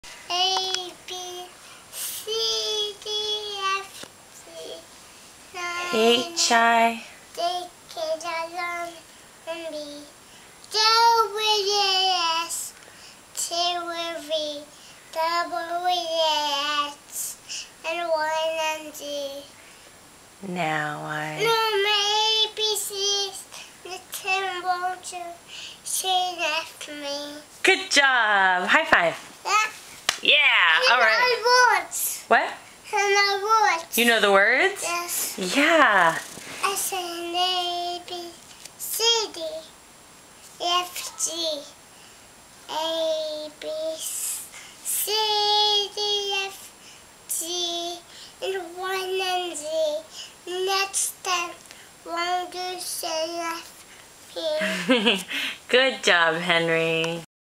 ABCs!
Henry (28 months old) is learning his ABCs!